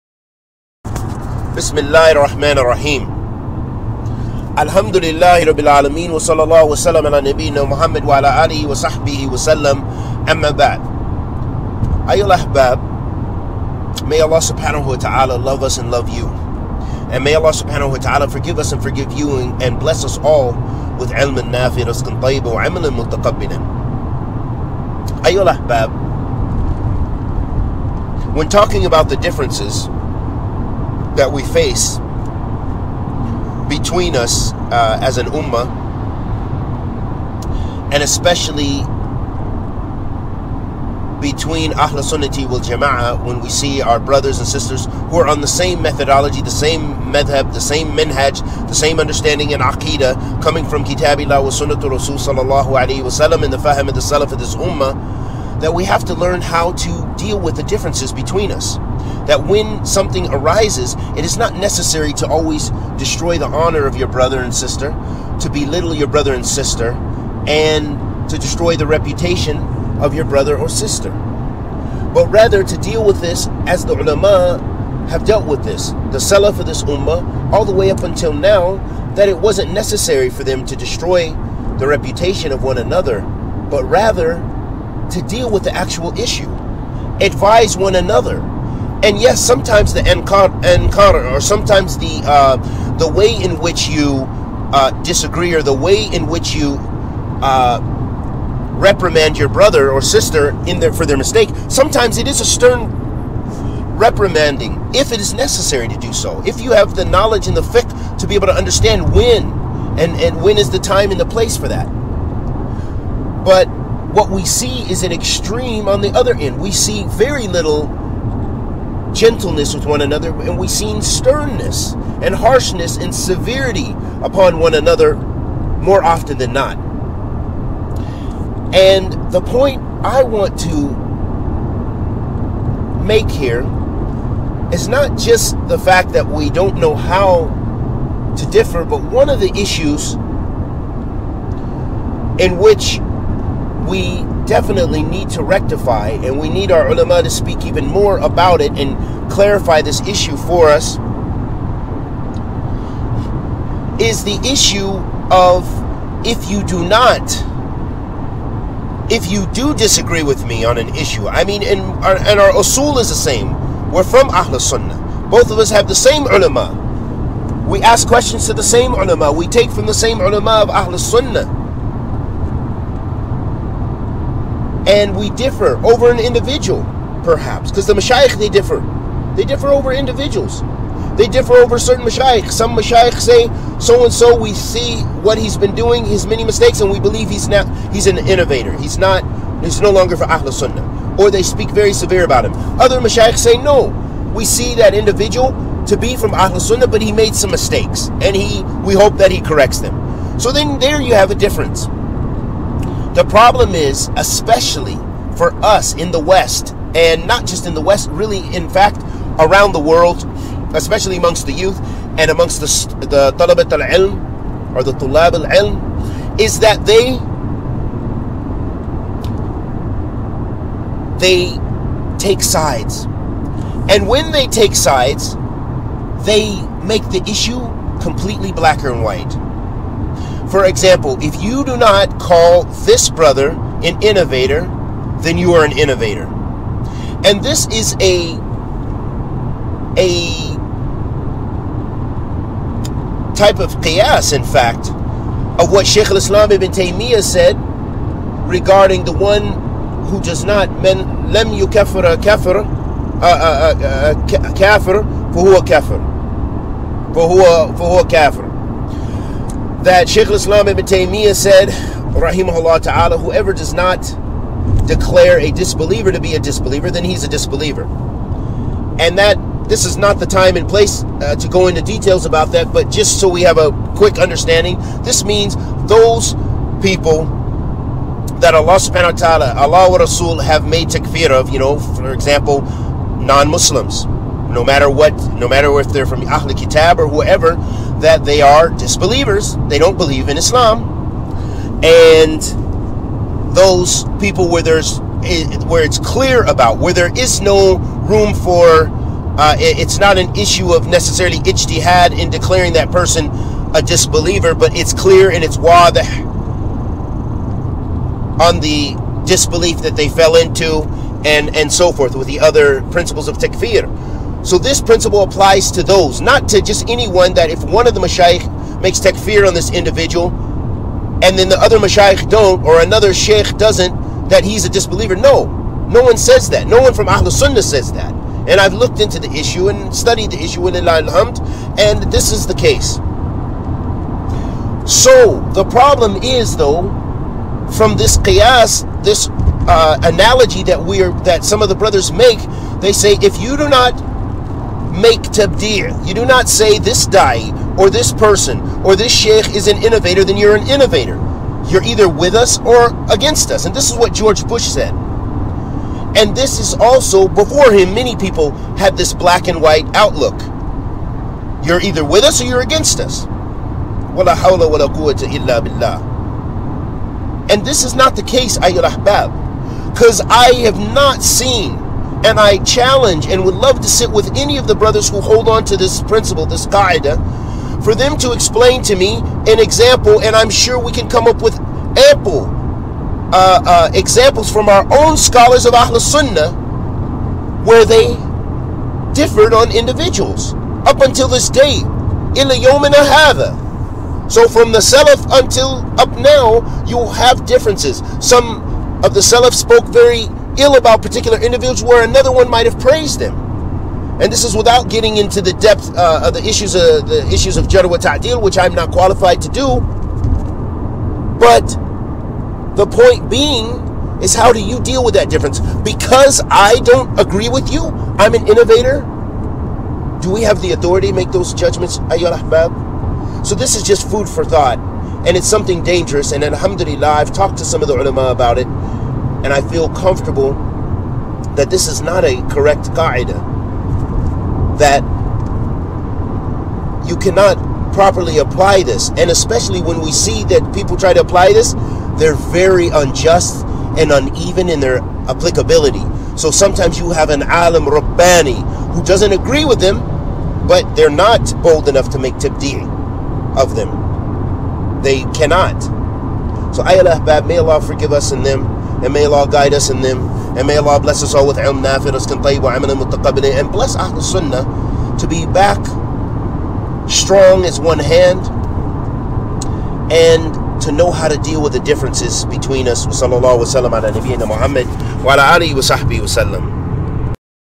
Learning How To Differ Without Destroying Each Other
Etiquettes of differing between Ahl Sunnah